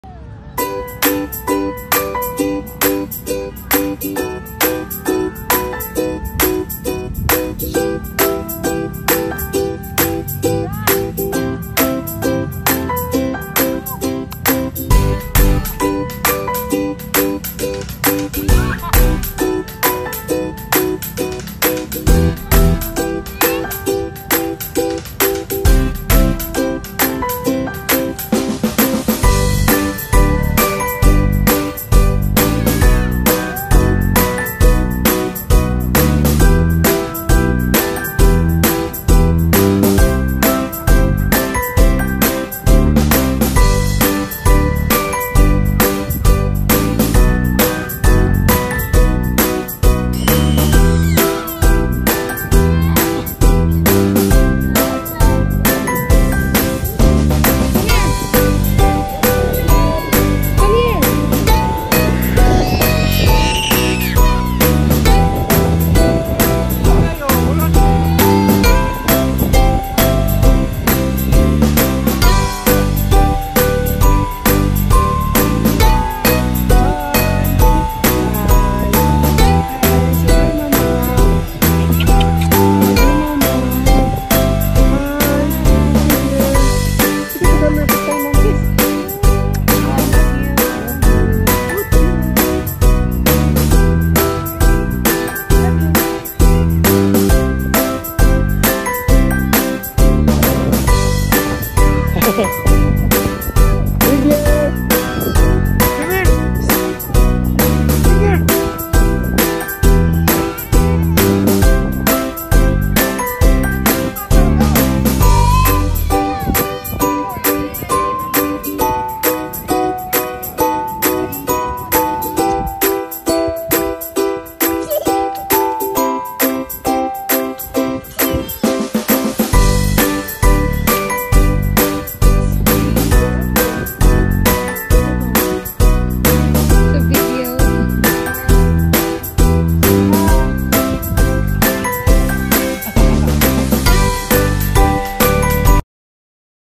My little boy first running on the green grass field sunset light.
It’s a beautiful day with love love!
🎥May 28-2023